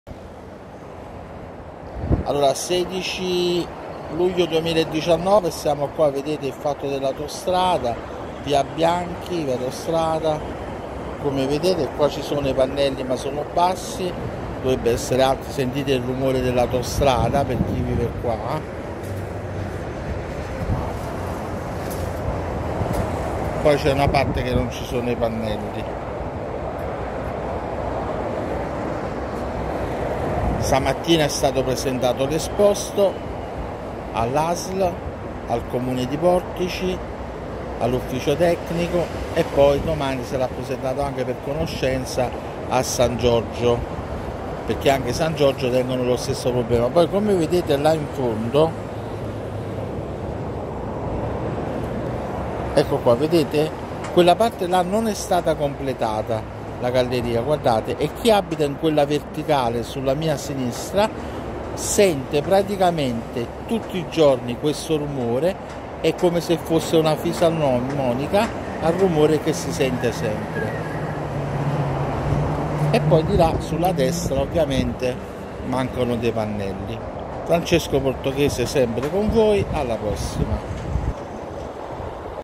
16 luglio 2019 via Libertà Portici società autostrade s p a
16 luglio 2019 è stata presentata la petizione popolare con esposto contro la società autostrade s.p.a. probabile inquinamento acustico atmosferico e ambientale.
Io ho messo la fiducia al Sindaco di Portici Cuomo V. l'assessore Iacone G. e l'ufficio Tecnico al dirigente Improta G